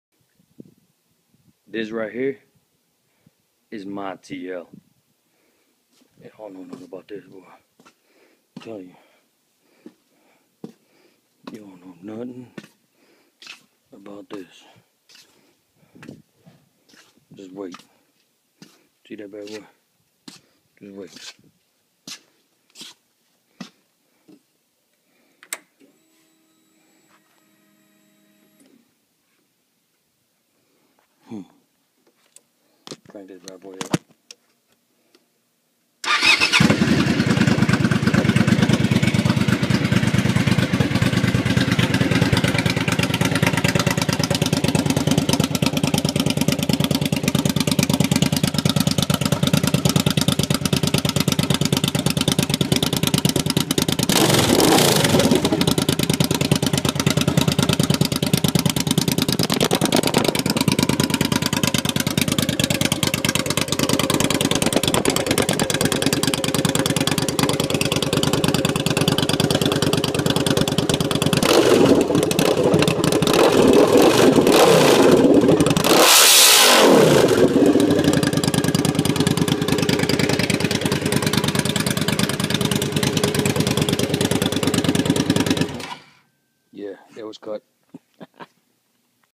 TL 1000R cut pipes
Just cut the pipes... Sounds sick!